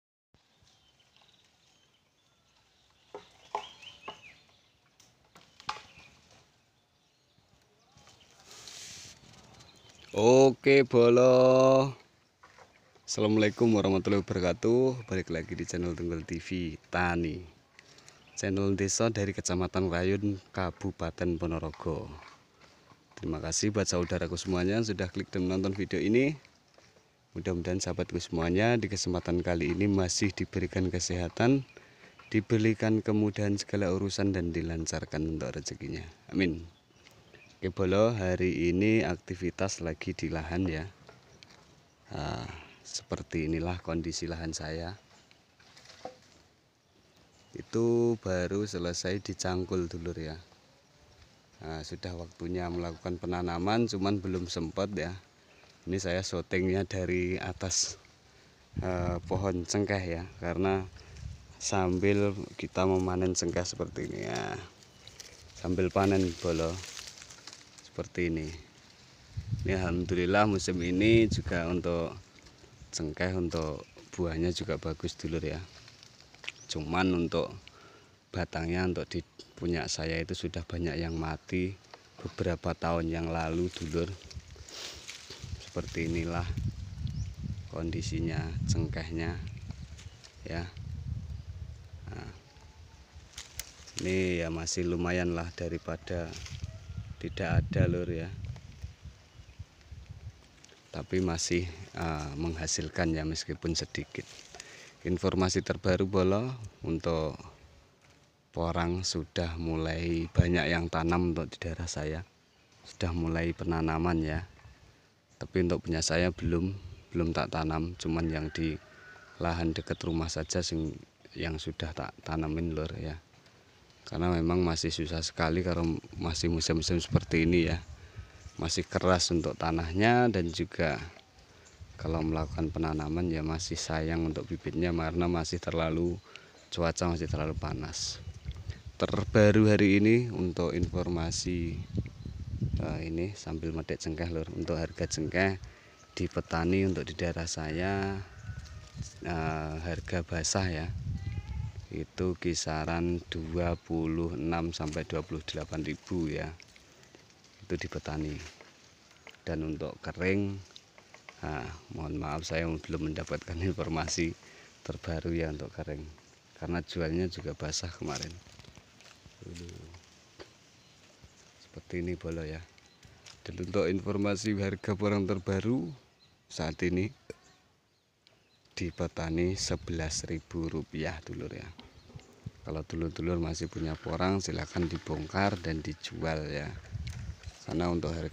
Berkebun memanen Cengkeh - info harga porang hari ini
bertani dipelosok desa. memanen cengkeh sambil bahas porang.